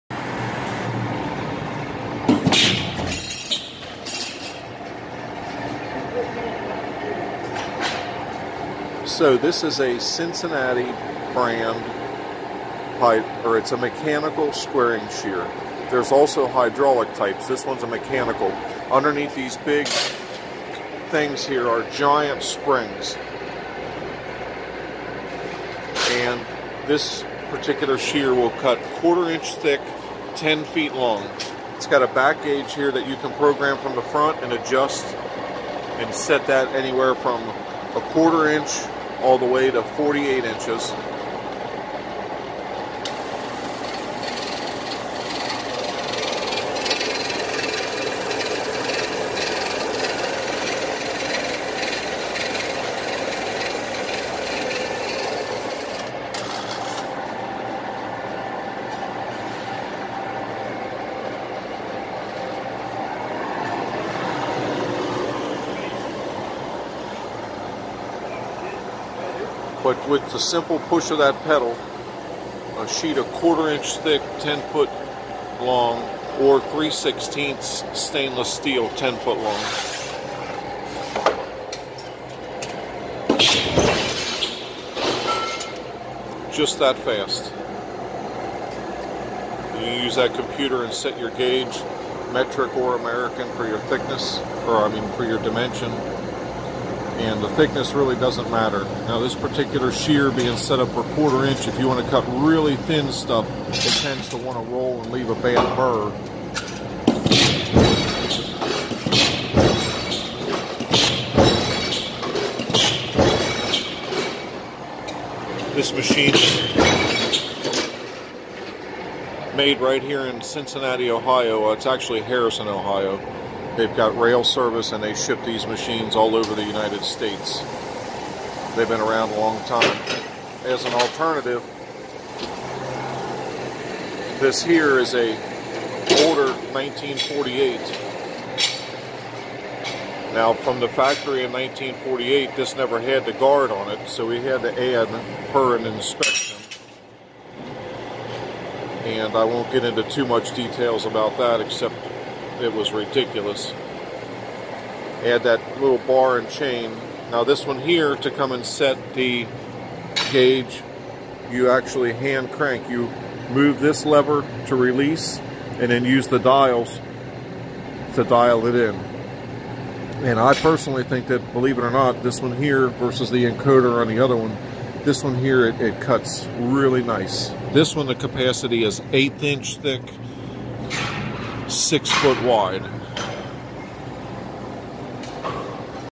Shearing sheet metal and plate
These are mechanical squaring sheers made by Cincinnati Incorporated on Kilby Road in Harrison Ohio They will cut up to 1/4 inch thick mild steel 10' long in about 1 second. We use them every day at our shop.